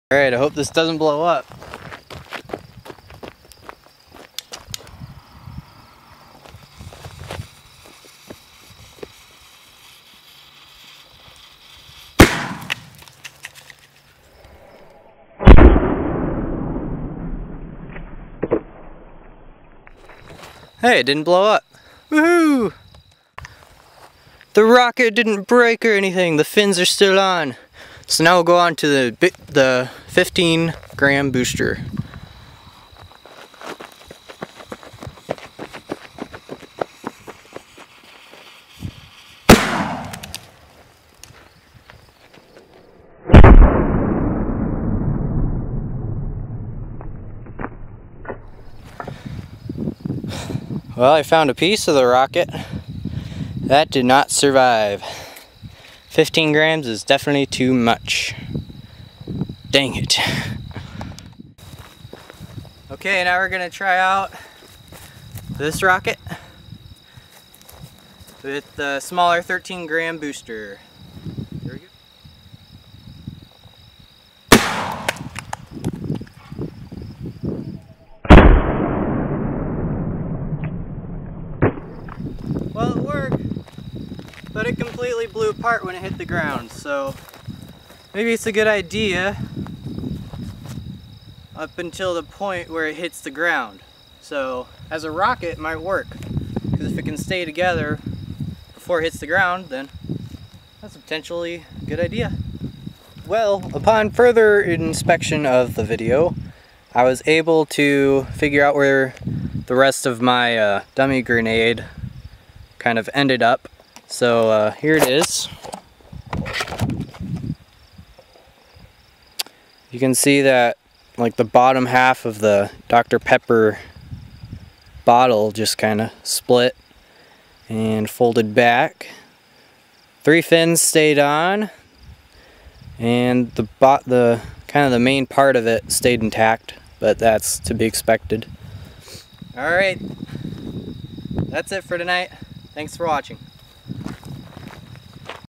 Dummy Rocket Tests with RPG-2
Trying out my new designs for rockets.  2 grams makes quite a difference!  I'll figure it out one of these times :)